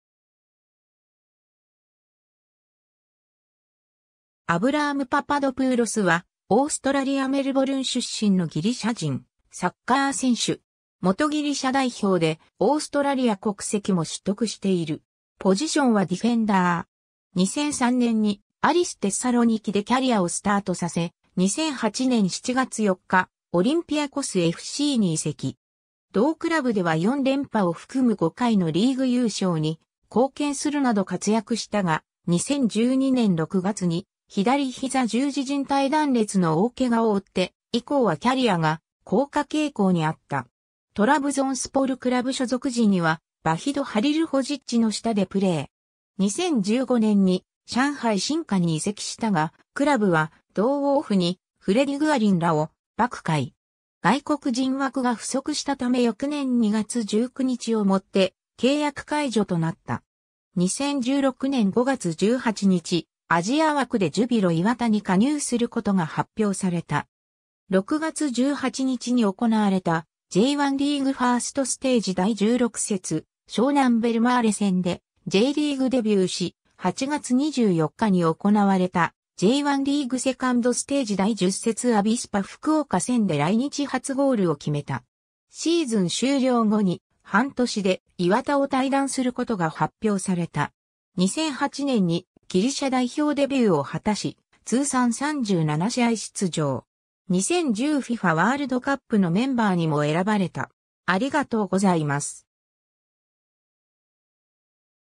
アヴラアム・パパドプーロス
アリス・テッサロニキの選手
トラブゾンスポル・クラブ_(サッカー)_の選手
ブリスベン・ロアーFCの選手
アヴラアム・パパドプーロス（Avraam Papadopoulos, Αβραάμ Παπαδόπουλος, 1984年12月3日 - ）は、オーストラリア・メルボルン出身のギリシャ人サッカー選手。
元ギリシャ代表で、オーストラリア国籍も取得している。
 2003年にアリス・テッサロニキでキャリアをスタートさせ、2008年7月4日、オリンピアコスFCに移籍。
同クラブでは4連覇を含む5回のリーグ優勝に貢献するなど活躍したが、2012年6月に左膝十字靭帯断裂の大怪我を負って以降はキャリアが降下傾向にあった。
トラブゾンスポル・クラブ所属時にはヴァヒド・ハリルホジッチの下でプレー。
 2015年に上海申花に移籍したが、クラブは同オフにフレディ・グアリンらを『爆買い』。
外国人枠が不足したため翌年2月19日を以て契約解除となった。
 2016年5月18日、アジア枠でジュビロ磐田に加入することが発表された。
6月18日に行われたJ1リーグ1stステージ第16節・湘南ベルマーレ戦でJリーグデビューし、8月24日に行われたJ1リーグ2ndステージ第10節・アビスパ福岡戦で来日初ゴールを決めた。
シーズン終了後に、半年で磐田を退団する事が発表された。
 2008年にギリシャ代表デビューを果たし、通算37試合出場。
2010 FIFAワールドカップのメンバーにも選ばれた。